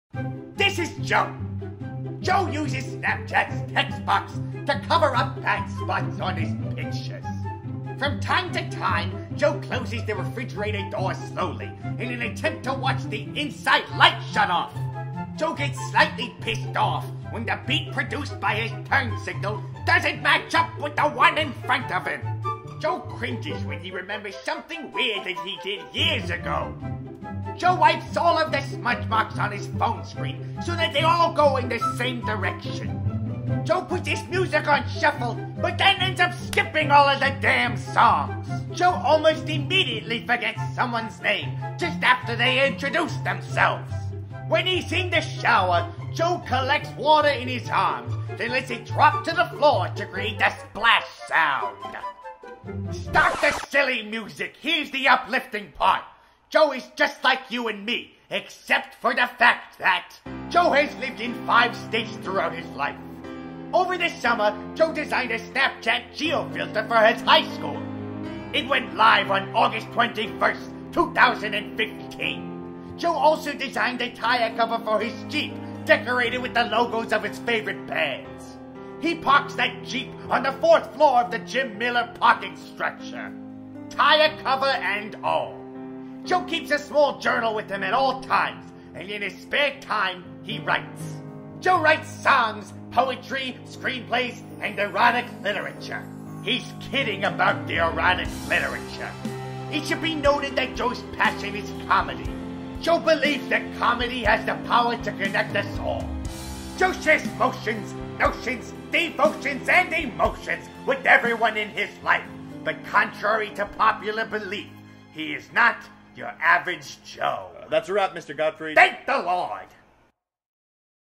Your Average Joe
Chapman University, Introduction to Visual Storytelling, September 2015

Joe is just like you and me...but he is NOT your average Joe. Enjoy!

Special thanks to:
Jake Cherry
Justine Winans
Rob Falk